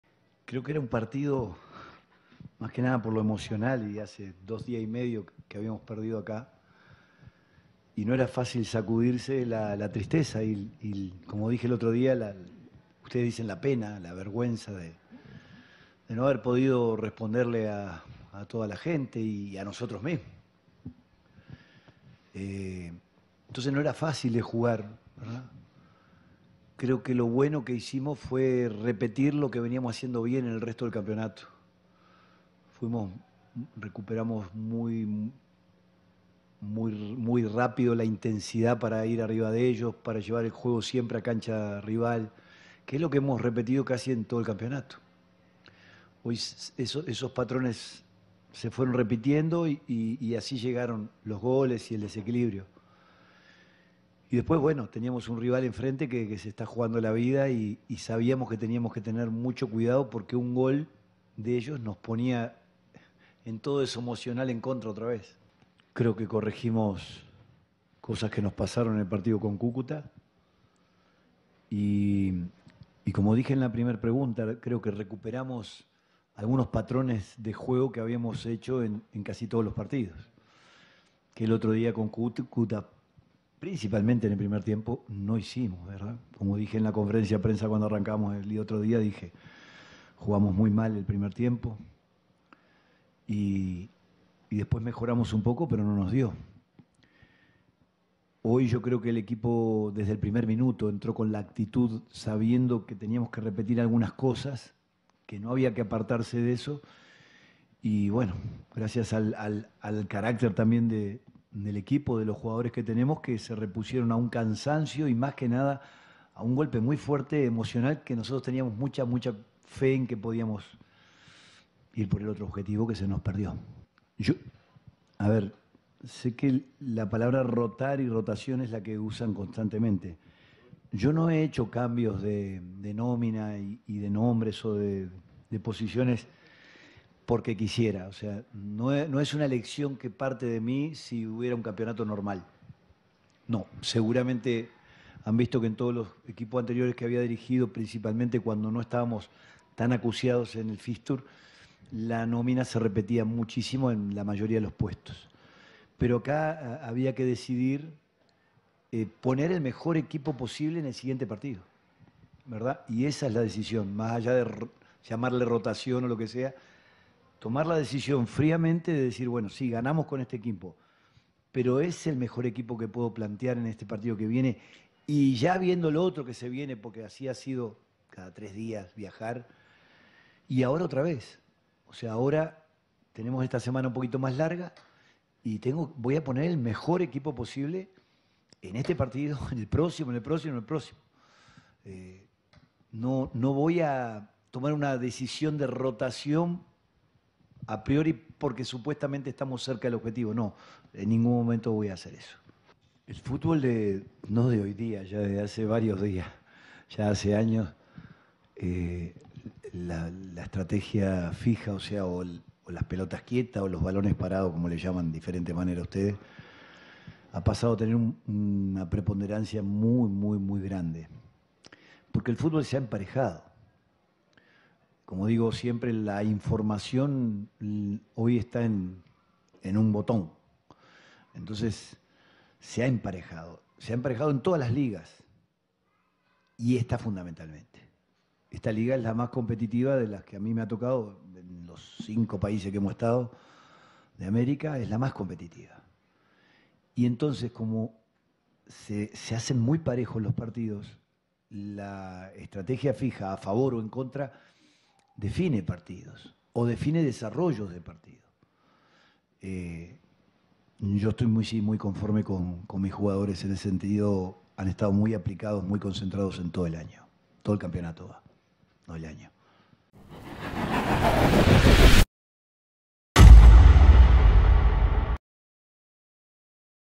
“NO voy a ROTAR la nómina solo por estar CERCA del OBJETIVO" Alfredo Arias 👀
El DIM llegó a 29 puntos y la clasificación a los 8 está cada vez más cerca, el equipo de Arias enfrentará el clásico ante Nacional la próxima fecha y no está entre sus planes rotar la titular.